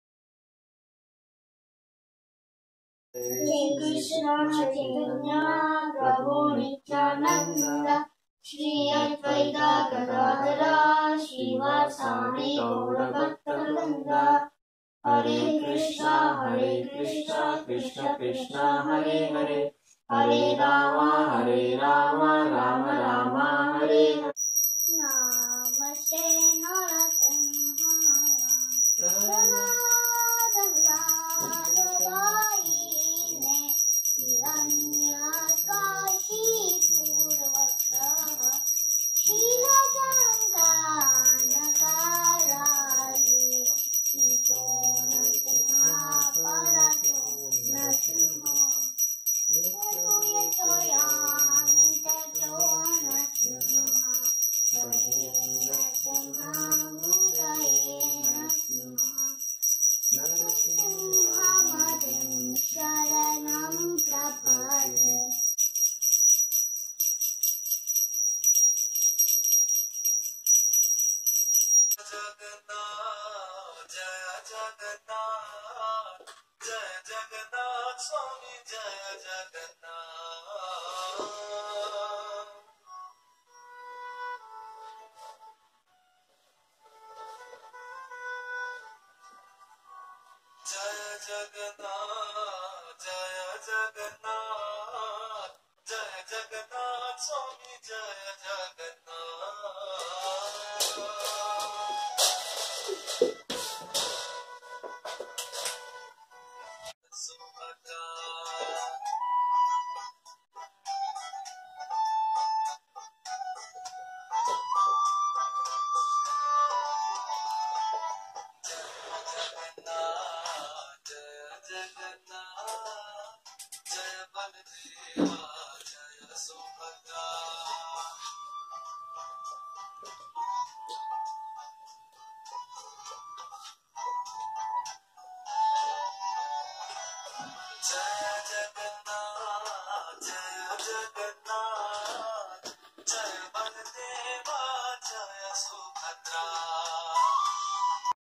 Bhargavi Mataj 2 minutes videoi Jagannath Ratha Yatra 2020
Devotees celebrating Jagannath Ratha Yatra 2020 at their home and taking blessing of Lord.